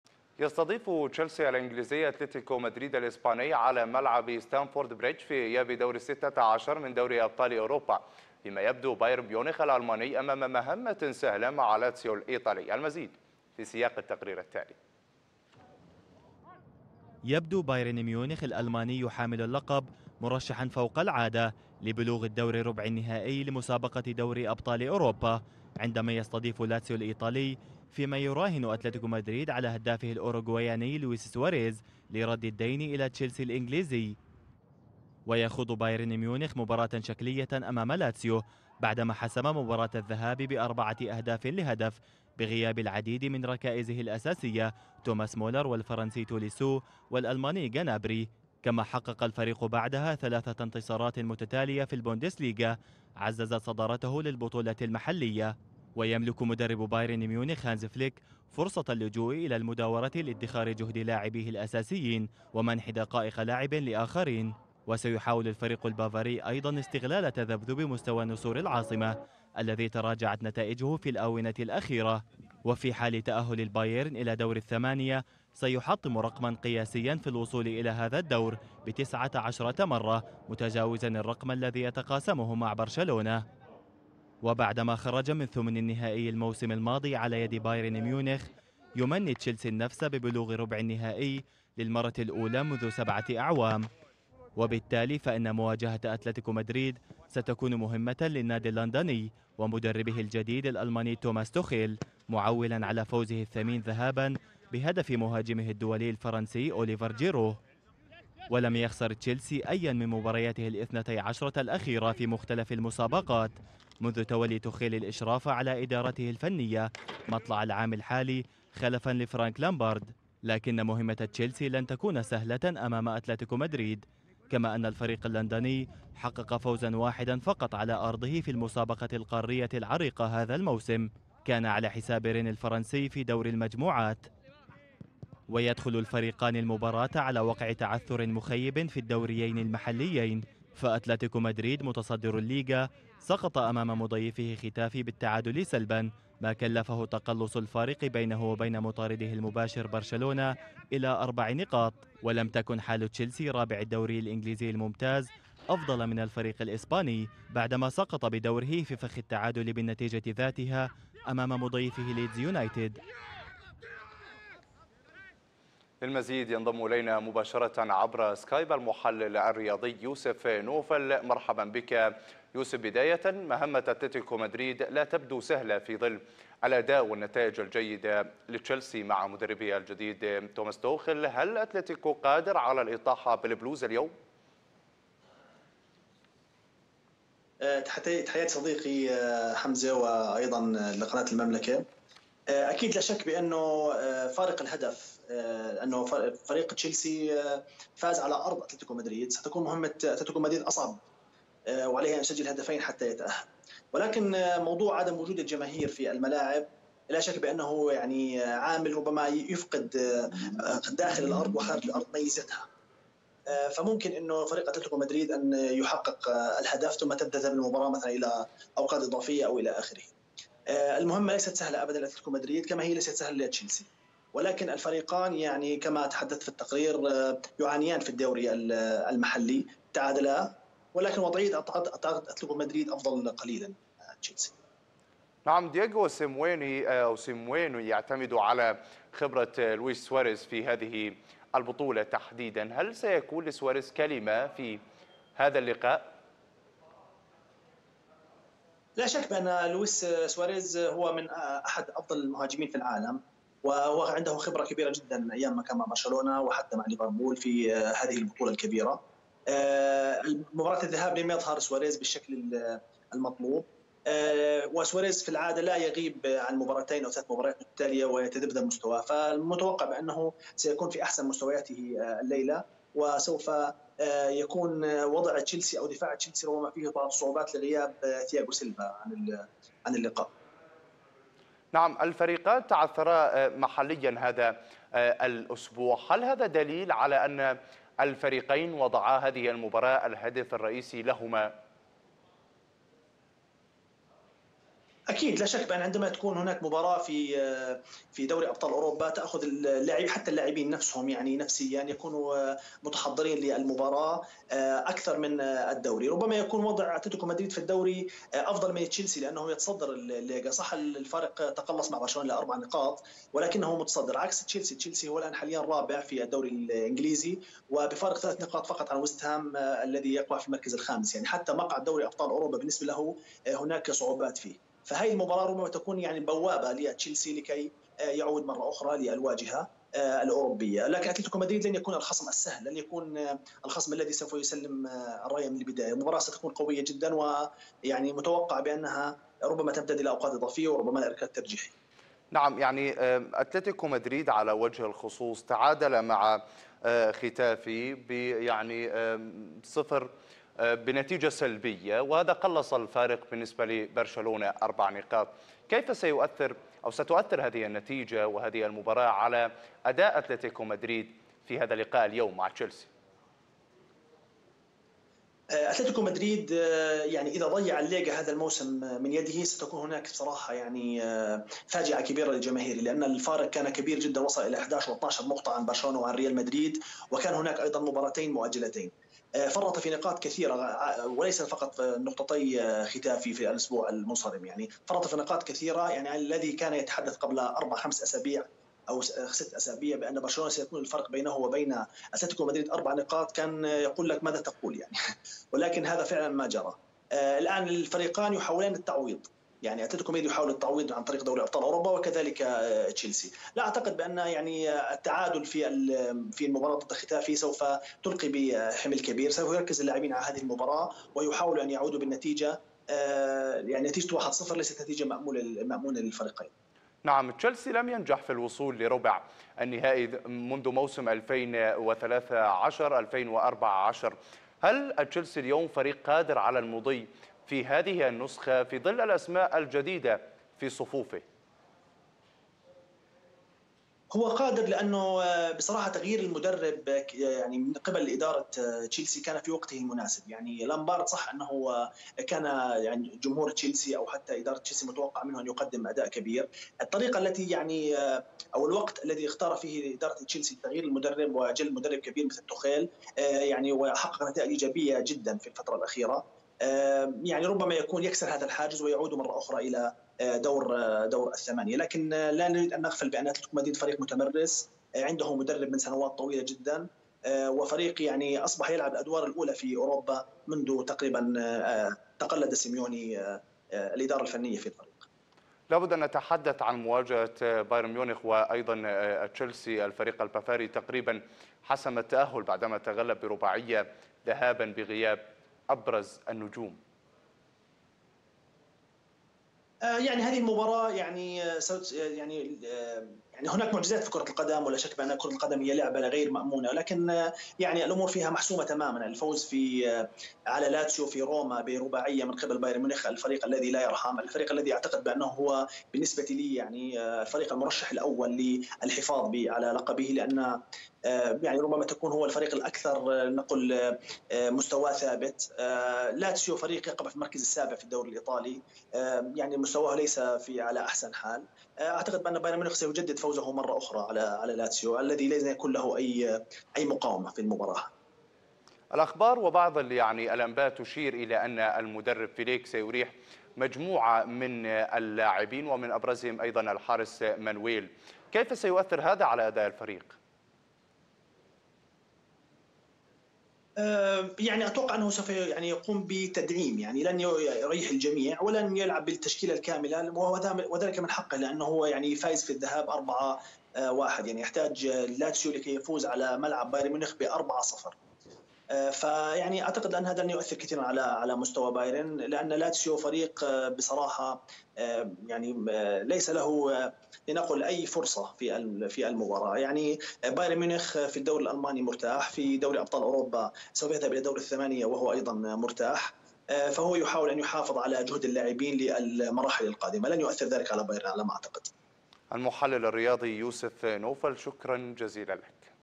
تشيلسي وأتلتيكو مدريد.. منافسة على بطاقة التأهل صوب دور الثمانية
المحلل الرياضي يوسف نوفل

هنا_المملكة